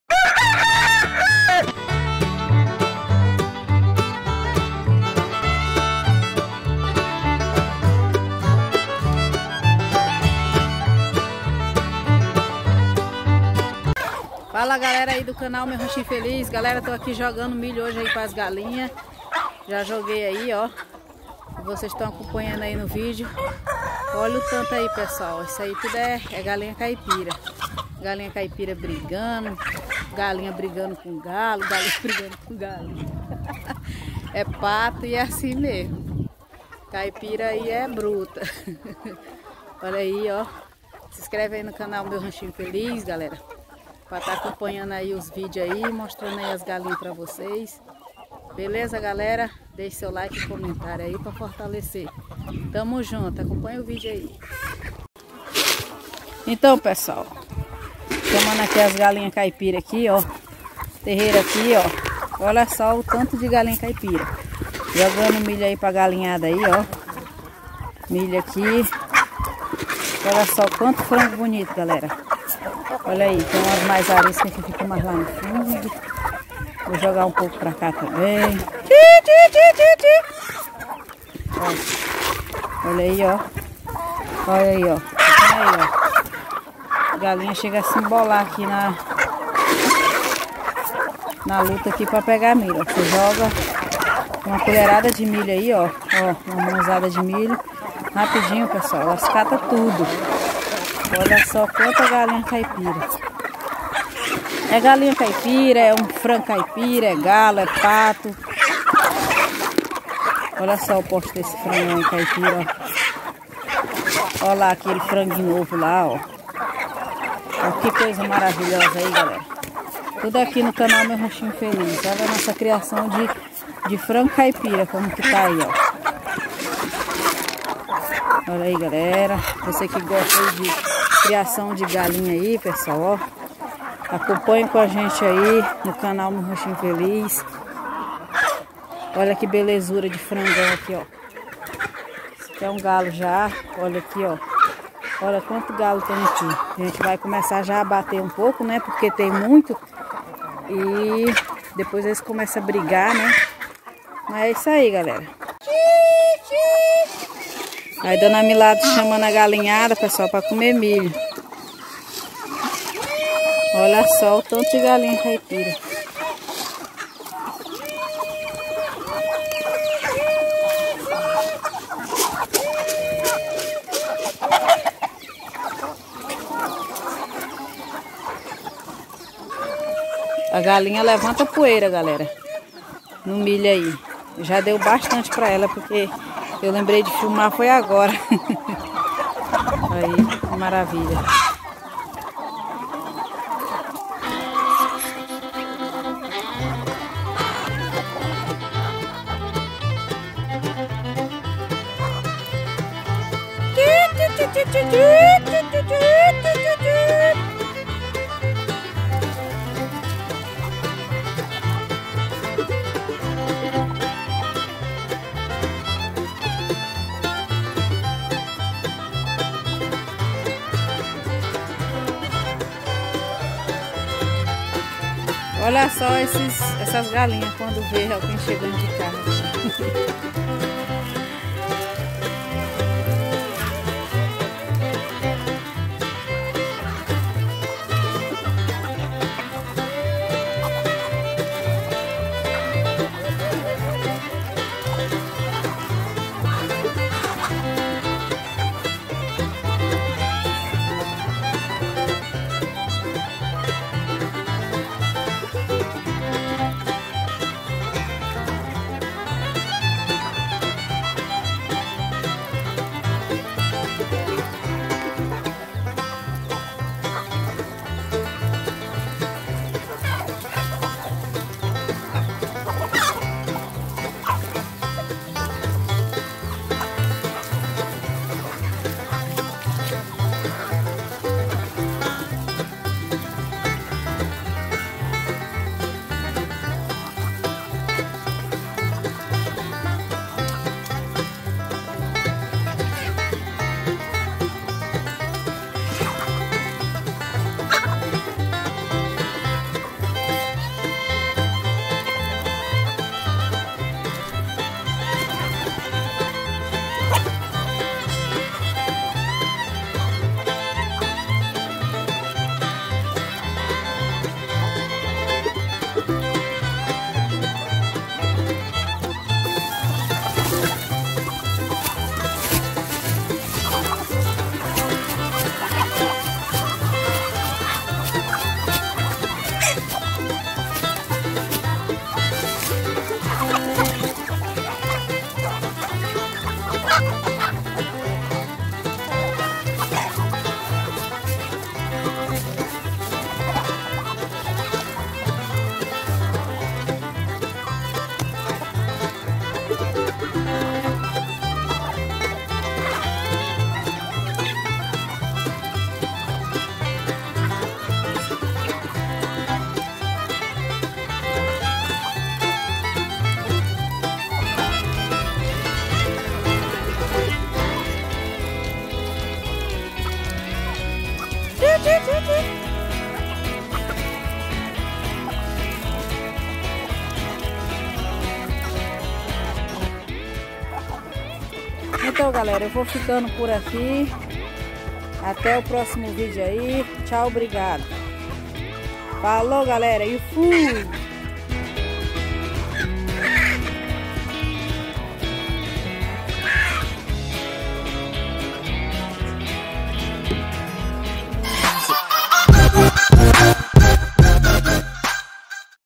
Criação de Galinhas,Frangos e Patos Caipiras.
Olá galera do canal Meu ranchinho feliz hoje mais um vídeo no canal onde vou mostrar a criação de Galinhas,Frangos e Patos caipiras.